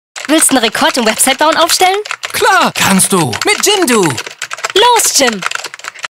Jimdo - Der Rekord - 3D Animation Werbung
Jimdo Werbespot mit Jim und Do.
Animation: The Soulcage Department
Produktion: Brickbeach